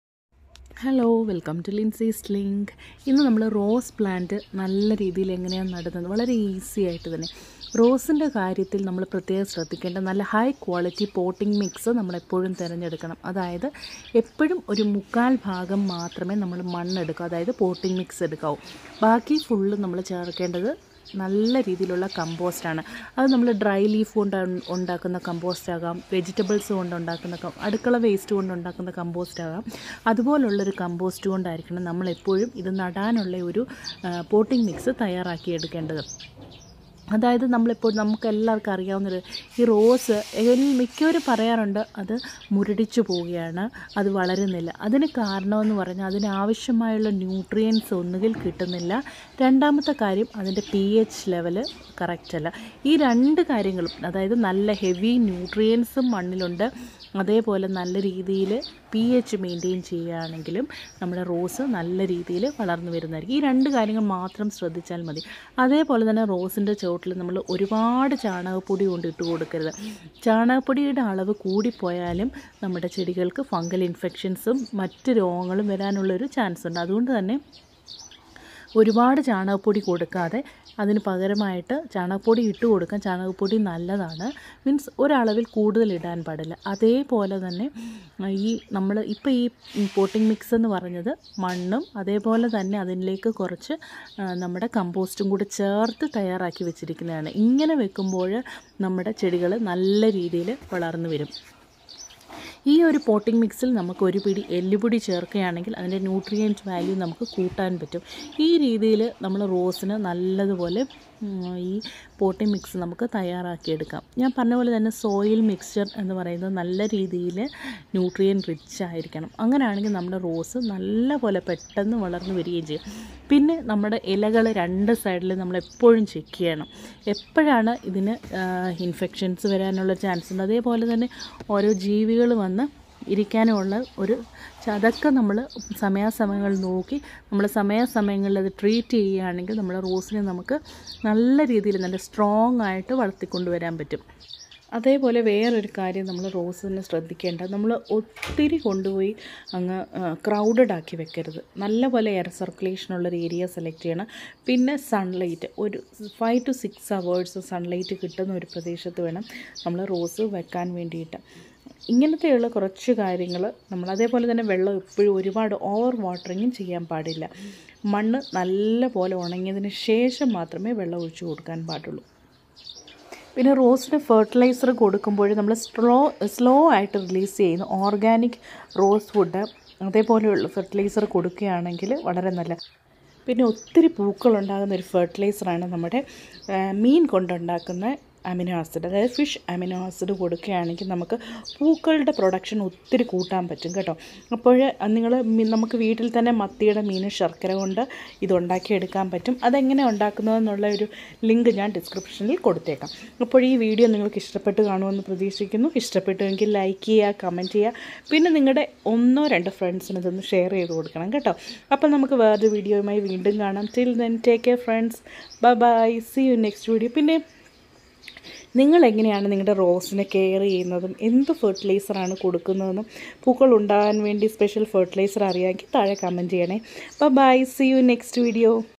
5 Secret Tips to Grow Rose Plants l Potting mix l Get more Rose flowers
Best method of Growing Rose Plants Bushy l Potting mix l Get more Rose flowers